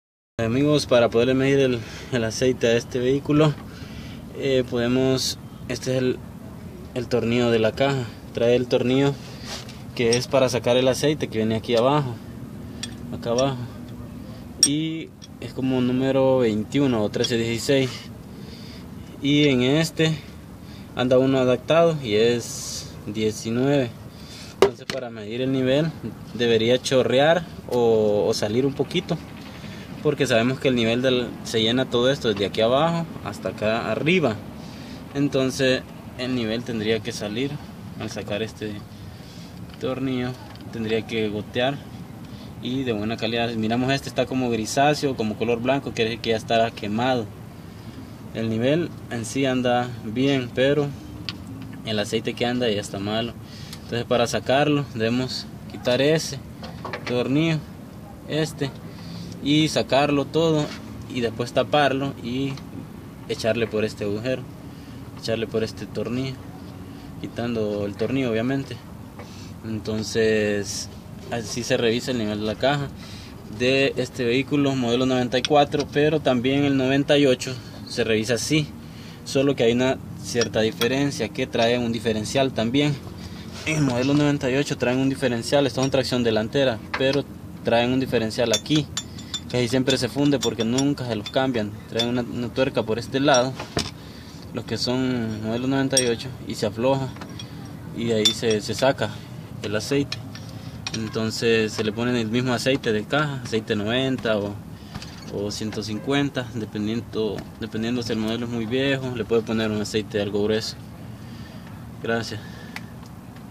Como Medir Nivel de Aceite Transmisión Mecánica Corolla👨‍🔧
🚕📞BRINDAMOS AYUDA GRATIS🌎, WE GIVE HELP FREE 💸
WEB TUTORIALES Nivel de aceite de la caja de transmisión corolla 94 al 97 ya que del 98 en adelante tienen diferencial también, osea que se deberán medir dos niveles en la transmisión. Este del diferencial muchas veces se pasa por alto pero es necesario cambiarlo sino la transmisión se hecha a perder.